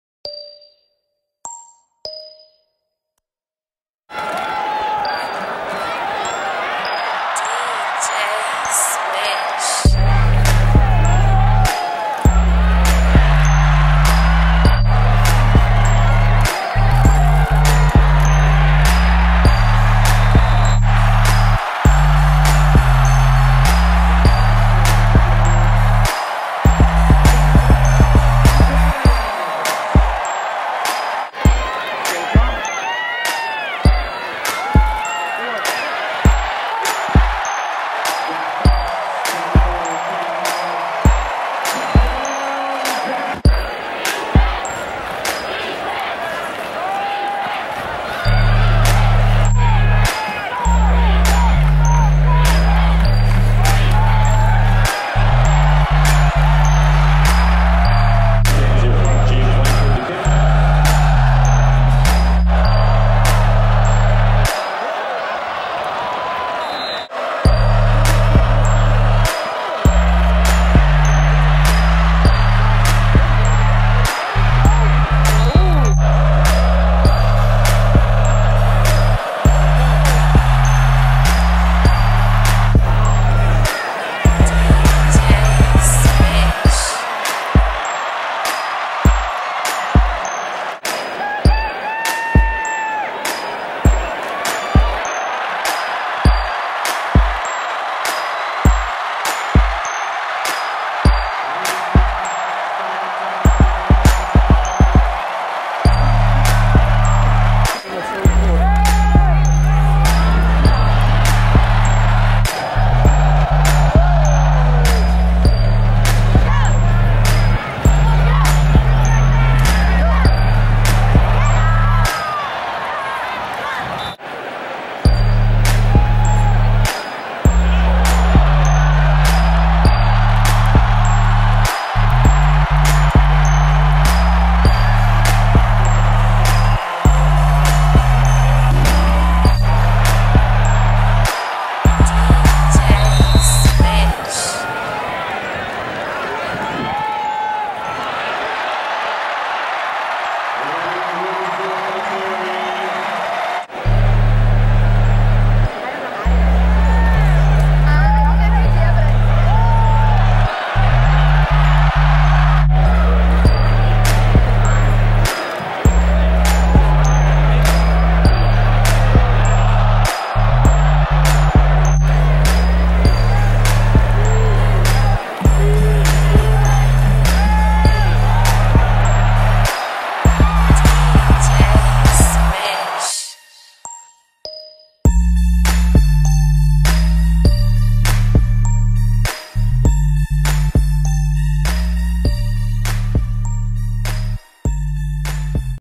Cal Blows Out Sacramento State, 55-14
Cal cruises to a 55-14 win over Sacramento State. The Bears are now 2-0 heading into their bye week, they will face off against the University of Arizona on the 19th of September at Arizona.